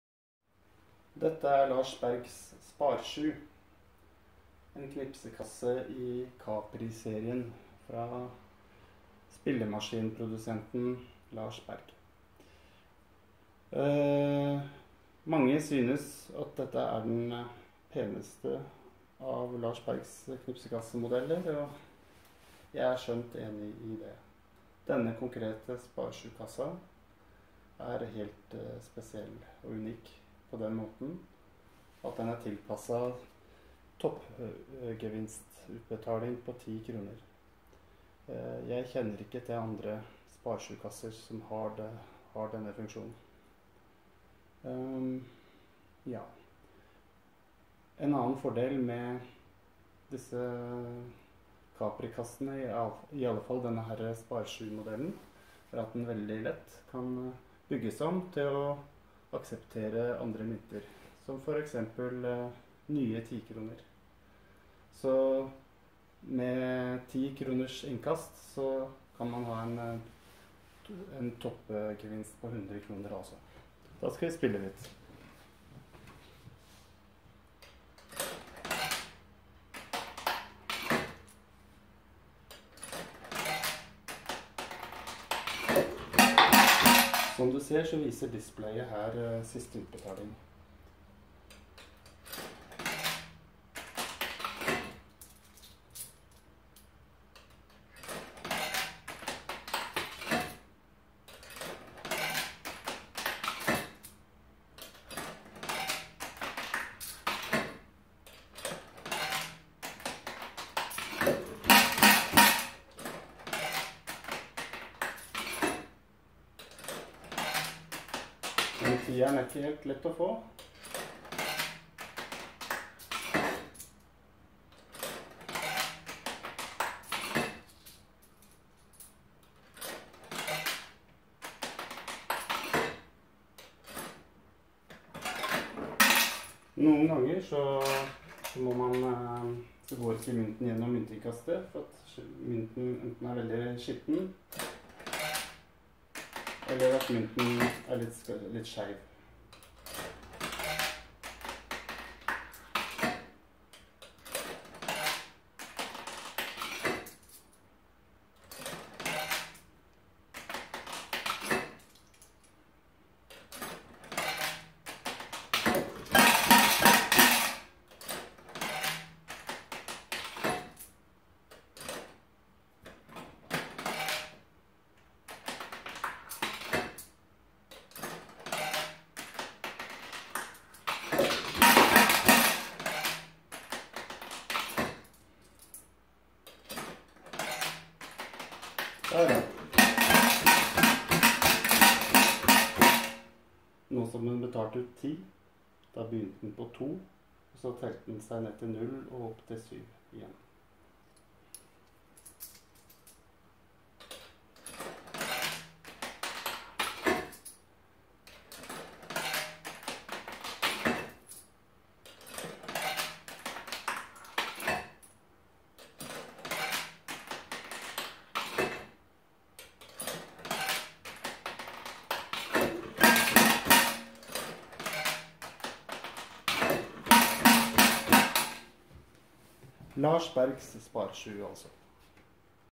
Spar 7 kronespill / knipsekasse med toppgevinst 10 kr !
Produsert av Lars Berg på slutten av 70 / tidlig 1980-tallet.
Spar 7 var tilpasset "de gamle" 1 -kr myntene. Knipsekassen kan enkelt tilpasses f.eks dagens 10-kr-mynter.

Under siste del av 1980-tallet; ble det stadig vanligere at toppgevinsten på knipsekassene ble økt fra 7 til 10 kr.

Dette er den eneste Spar 7 -maskinen jeg vet om; som har denne tilpasningen. Toppgevinsten er mao. 10 kr; og ikke 7 kr.